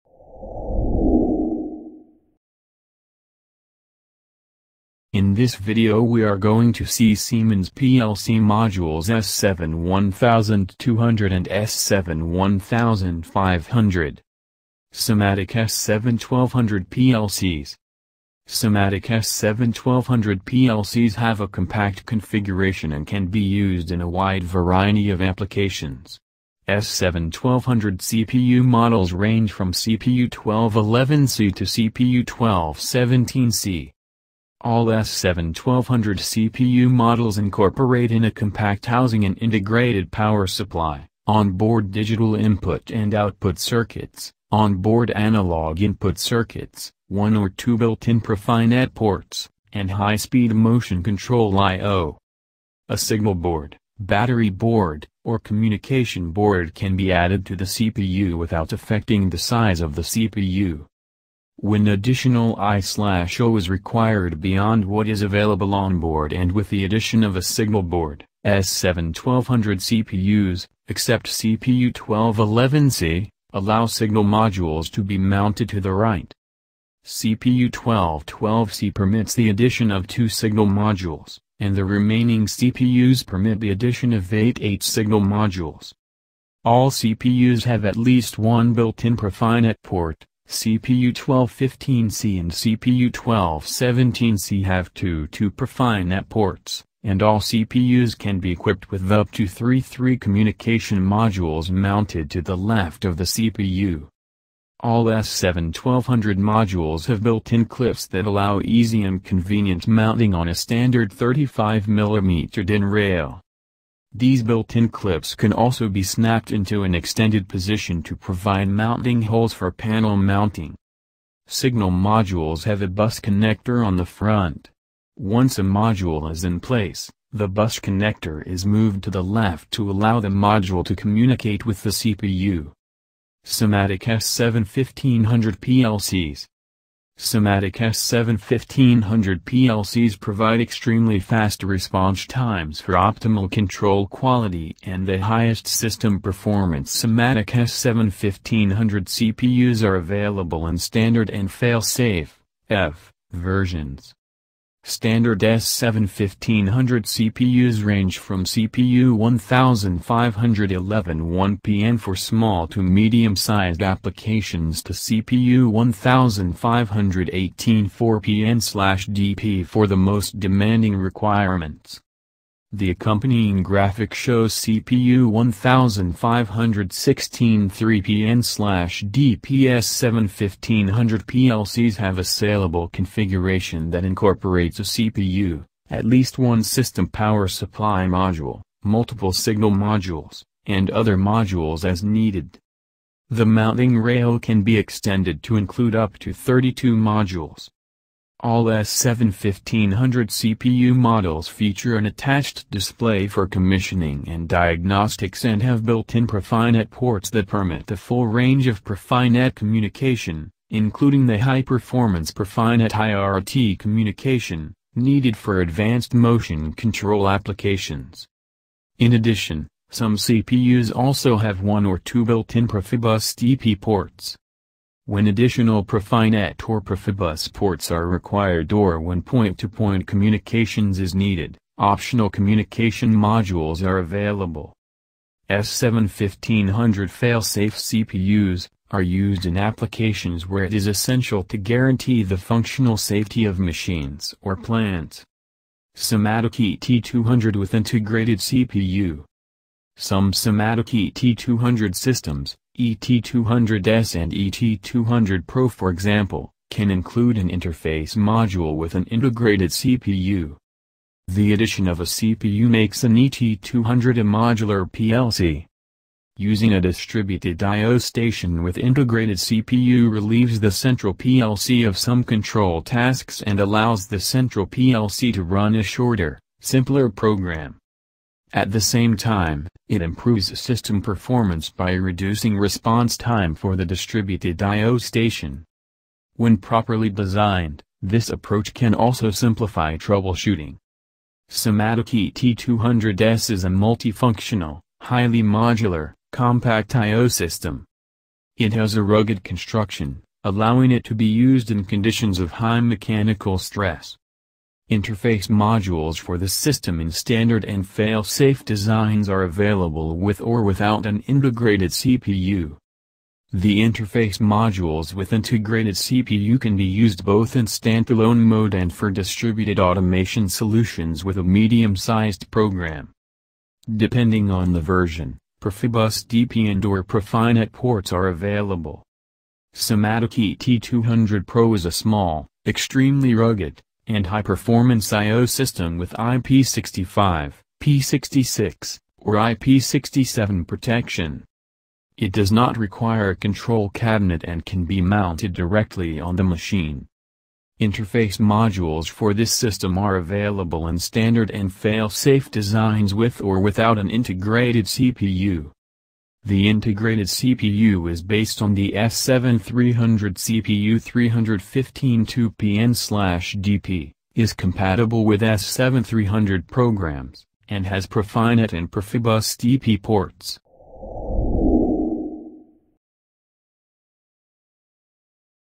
Siemens Simatic PLC Module S7-1200 and S7-1500 Controller
In This Video We are going to see  Siemens Simatic PLC Module S7 1200 and S7 1500 Controller Overview.

SIMATIC S7-1200 PLC'S have a compact configuration and can be used in a wide variety of applications. S7-1200 CPU models range from CPU 1211C to CPU 1217C.

SIMATIC S7-1500 PLC'S provide extremely fast response times for optimal control quality and the highest system performance SIMATIC S7-1500 CPU'S are available in Standard and Fail safe (F) versions.

Thank you for viewing our Video, and have a wonderful day.